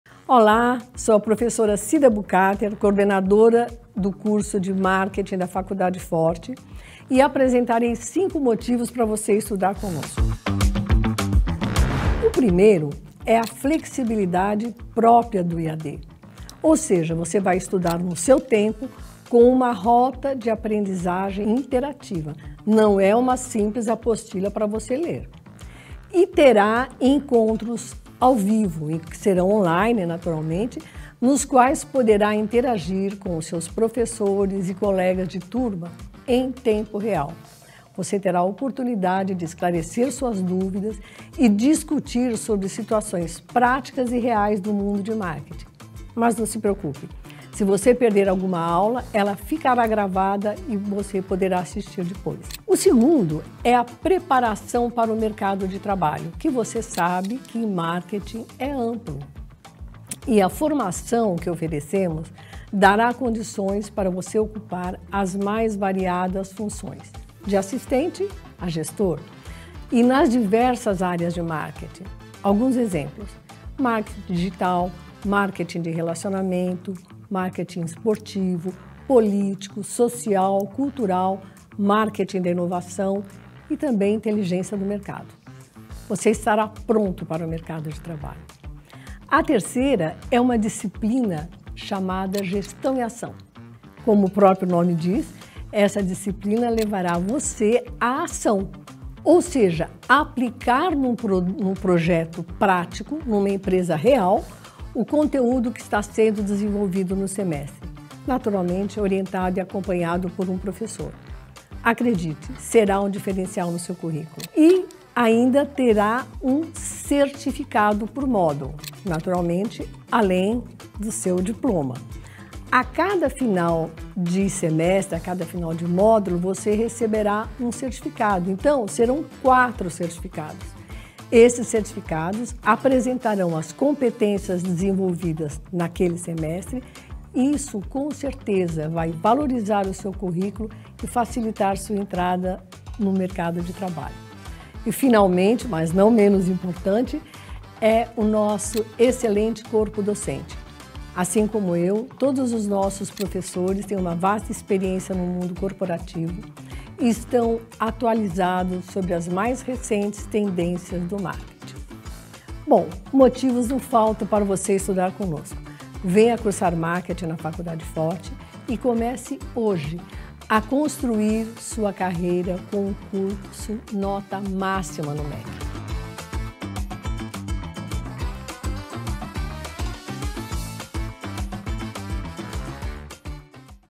5 Motivos para Estudar Marketing na Faculdade Phorte
Descubra as oportunidades de estudo no curso de Marketing da Faculdade Phorte com a professora Cida Bucater.
Flexibilidade e aprendizagem interativa são apenas o começo - este curso prepara você para diversas funções no mundo do marketing.
Saiba mais sobre as vantagens práticas, como projetos em empresas reais, certificados por módulo, e a experiência dos nossos professores.

Saiba mais sobre a Graduação em Marketing da Faculdade Phorte